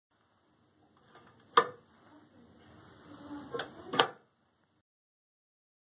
BeLock Energized
The BeLock Has Been Enhanced to Offer a Switch in the Housing.  Not Only Will You Have The Most Secure Sectional Door Lock Available But You Can Also Control:

Door Operator/Opener
Dock Leveler
Building Security System
Loading Light
Traffic Safety lights
Efficiency Tracking System

Any Other Device That You Want to Energize or De-Energize When You Unlock Your Dock Door.
Loading Dock Security and Safety is Taken to a New Level.

or